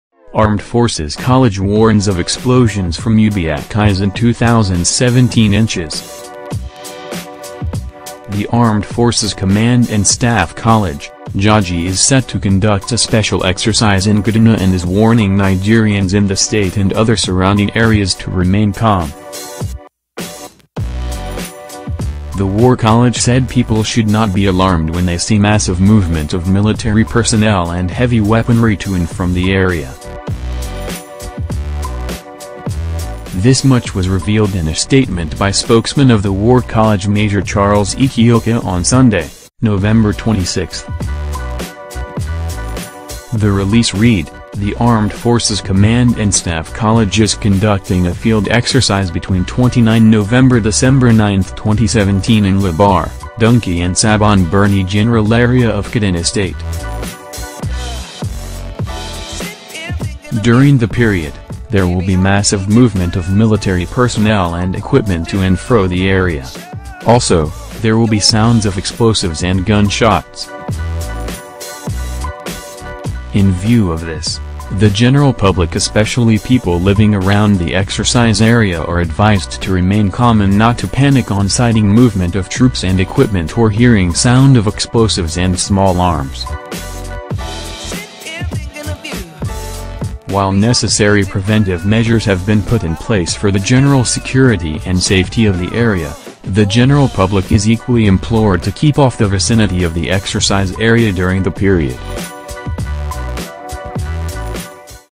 Armed Forces college warns of explosions from Ubiak-Isin 2017
- War college spokesman Major Ekeocha said residents of Kaduna and surrounding area should expect to hear explosions and gunshots but there was not need to panic

- The exercise Ubiak-Isin is the 2017 edition and will hold from Wednesday, November 29 to Saturday, December 9

- The college assures residents in the area of their safety because all necessary precautions have been taking